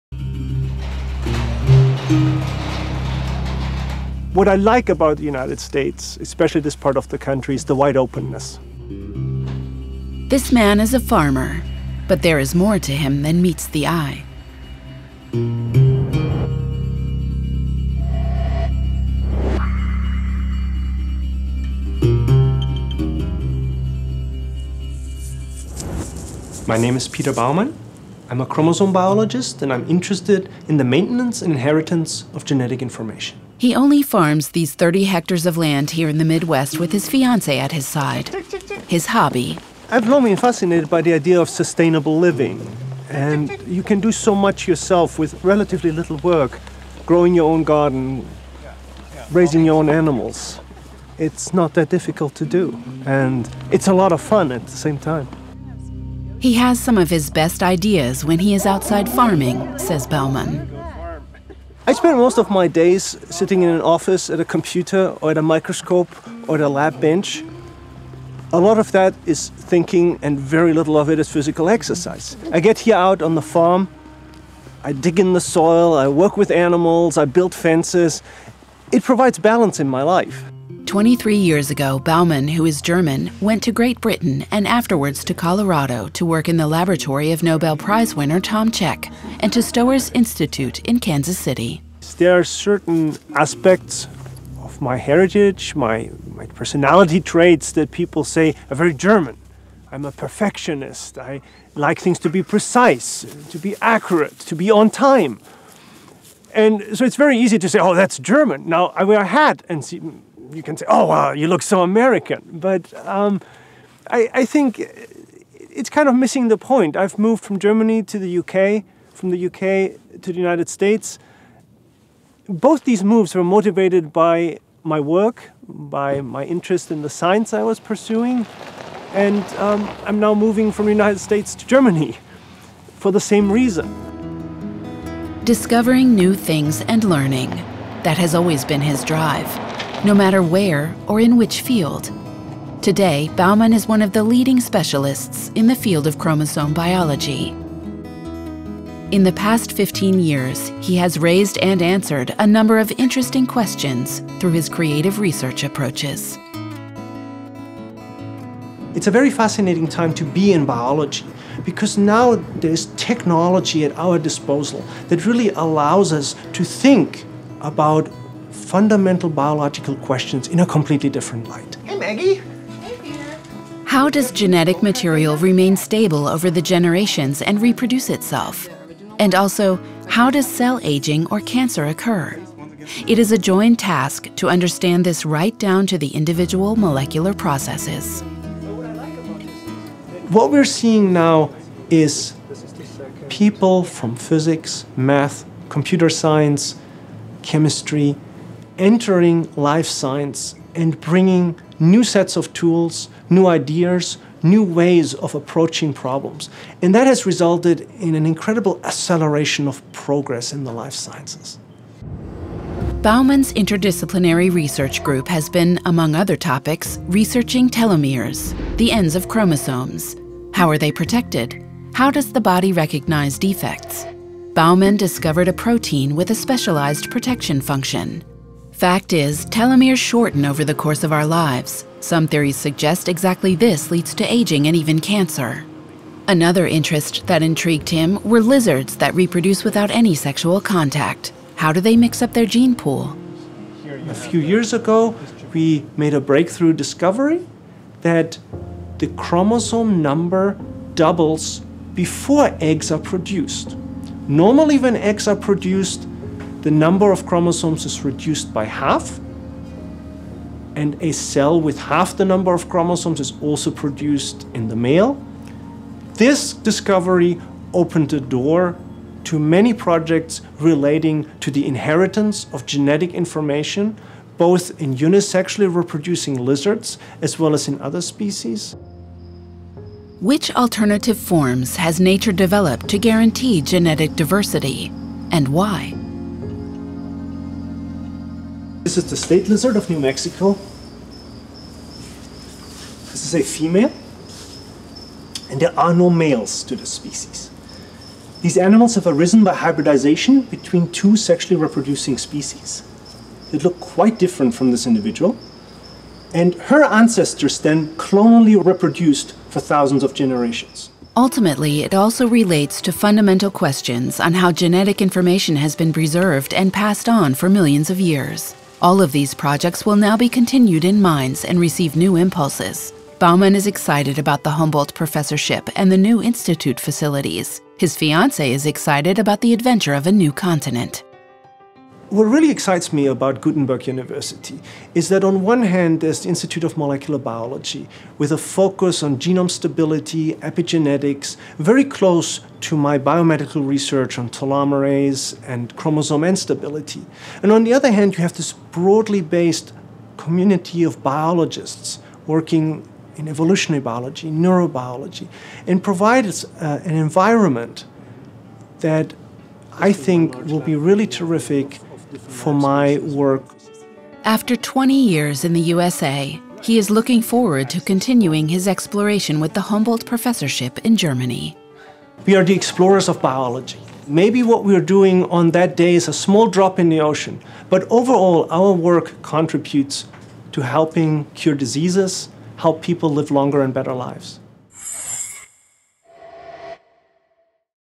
Peter Baumann - Alexander von Humboldt-Professorship 2017 (EN)
When cells divide, telomerase plays a key role, an enzyme that preserves the telomeres, the ends of the chromosomes, which get shorter with every cell division. Controlling telomerase determines whether cells age or whether, in the case of cancer, for example, they grow uninhibitedly. The cell biologist, Peter Baumann, is a leading specialist in the field of chromosome biology, genome stability and the regulation of telomerase and other molecular cell-ageing processes. His discoveries on the RNA components of telomerase (Terc) in yeast could lead to a better understanding of Terc regulation in human cells and facilitate therapies to counter early cell-ageing or pathological growth. In Mainz, Baumann will help to develop the university and the Institute of Molecular Biology (IMB) into an international location for fundamental research in biomedicine.